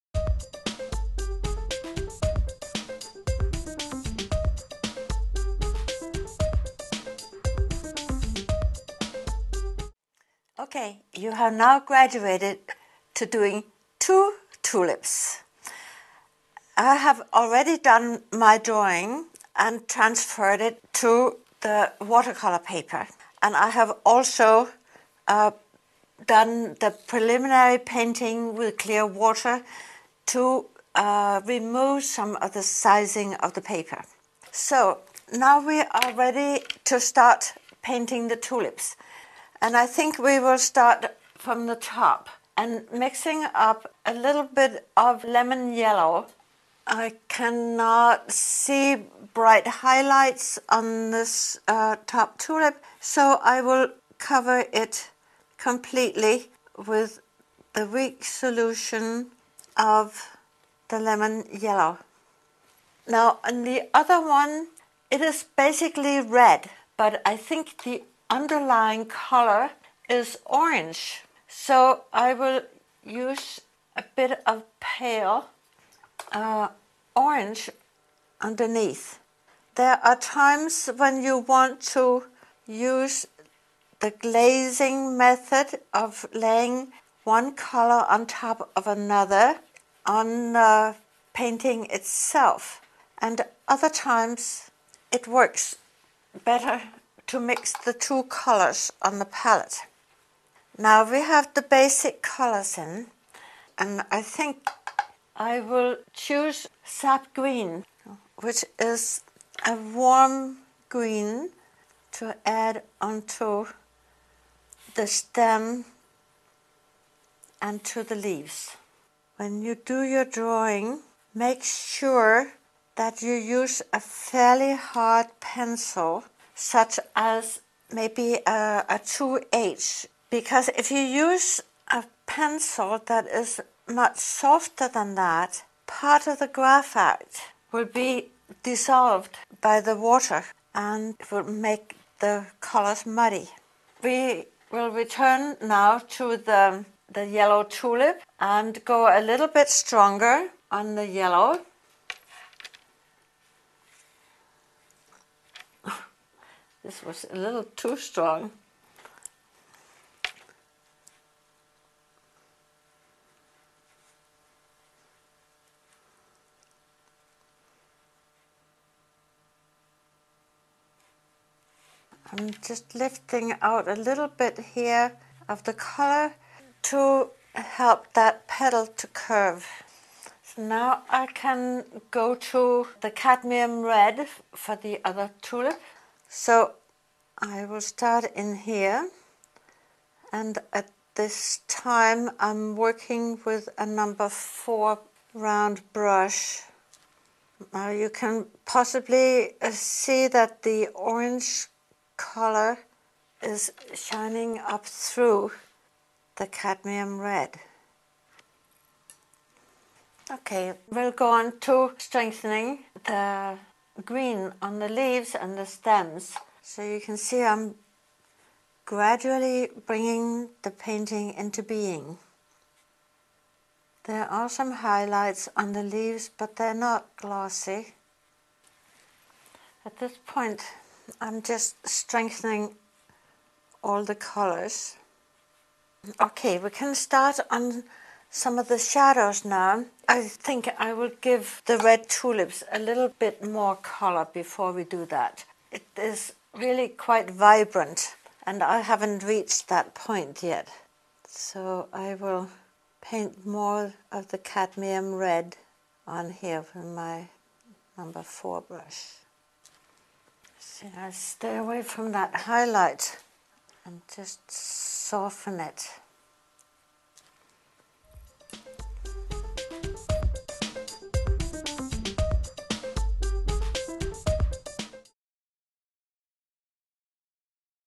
Preview | Beautiful Botanicals With Bente Starcke King
Working from fresh flowers and plants, Bente introduces the techniques and principles involved in botanical painting, in this video on ArtistsNetwork.tv. Using watercolor, colored pencil and ink, Bente shows you how to gain control of a painting and apply different media to great effect. In this workshop, Bente paints tulips, primroses, daffodils, and lilies with the addition of helpful hints and tips along the way, tackling potential problems such as white flowers on a white background. This video is a must for all who have an interest in painting beautiful flowers. See the whole video at ArtistsNetwork.tv!